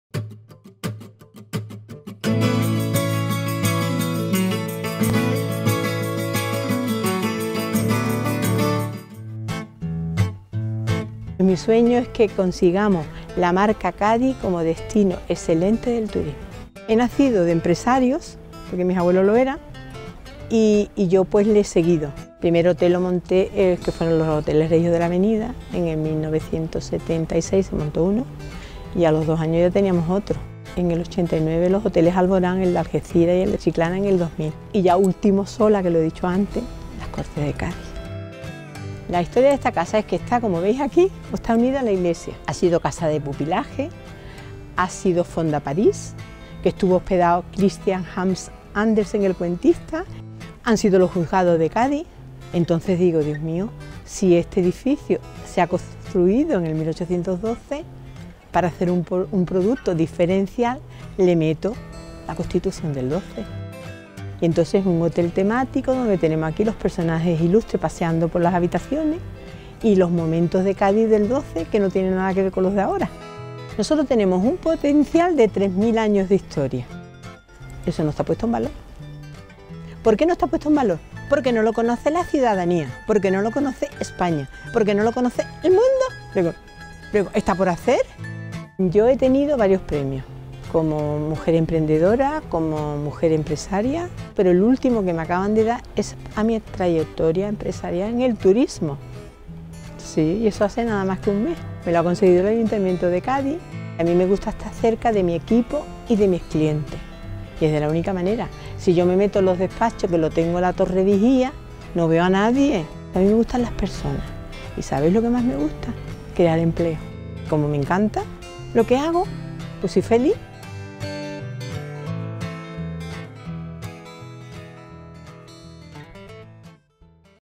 HDL Josefa Díaz, en busca de la excelencia turística de Cádiz con Las Cortes como atractivo
Lo que más le gusta, asegura, es el contacto con el público. Es por ello por lo que Josefa Díaz, propietaria y directora del Hotel Las Cortes de Cádiz, tiene su despacho a la vista de todos, en el patio central del edificio. Esta gaditana tiene un sueño: “Conseguir la marca Cádiz como destino excelente del turismo”. Y por  ese objetivo trabaja, no sólo en su negocio sino también como presidenta de la Asociación de Empresas Turísticas de Cádiz y del Skal Club. En 2014, ha recibido un reconocimiento del ayuntamiento de su ciudad por “toda una vida dedicada al turismo”.